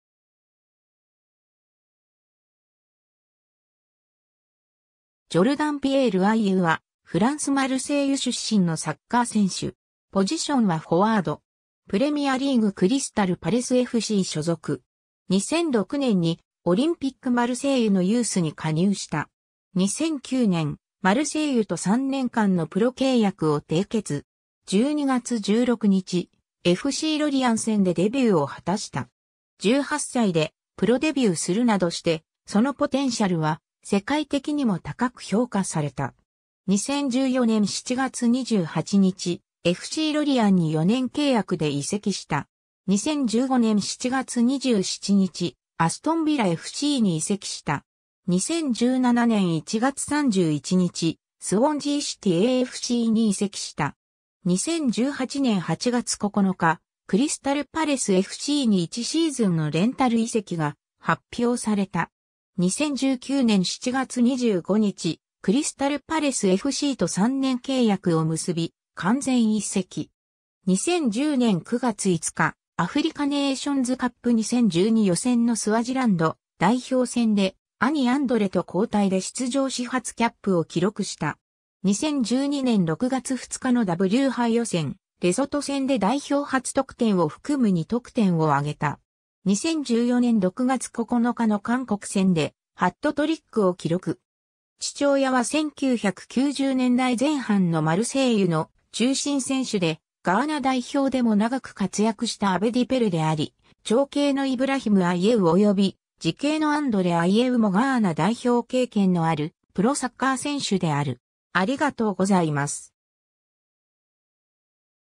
ジョルダン・アイェウ
オリンピック・マルセイユの選手
アストン・ヴィラFCの選手
スウォンジー・シティAFCの選手
クリスタル・パレスFCの選手
ジョルダン・ピエール・アイェウ（Jordan Pierre Ayew, 1991年9月11日 - ）は、フランス・マルセイユ出身のサッカー選手。
プレミアリーグ・クリスタル・パレスFC所属。
 2006年にオリンピック・マルセイユのユースに加入した。
2009年、マルセイユと3年間のプロ契約を締結。
12月16日、FCロリアン戦でデビューを果たした。
 18歳でプロデビューするなどして、そのポテンシャルは世界的にも高く評価された。
 2014年7月28日、FCロリアンに4年契約で移籍した。
 2015年7月27日、アストン・ヴィラFCに移籍した。
 2017年1月31日、スウォンジー・シティAFCに移籍した。
 2018年8月9日、クリスタル・パレスFCに1シーズンのレンタル移籍が発表された。
 2019年7月25日、クリスタル・パレスFCと3年契約を結び完全移籍。
 2010年9月5日、アフリカネイションズカップ2012予選のスワジランド代表戦で兄アンドレと交代で出場し初キャップを記録した。
2012年6月2日のW杯予選・レソト戦で代表初得点を含む2得点を挙げた。
2014年6月9日の韓国戦でハットトリックを記録。
 父親は1990年代前半のマルセイユの中心選手でガーナ代表でも長く活躍したアベディ・ペレであり、長兄のイブラヒム・アイェウ及び次兄のアンドレ・アイェウもガーナ代表経験のあるプロサッカー選手である。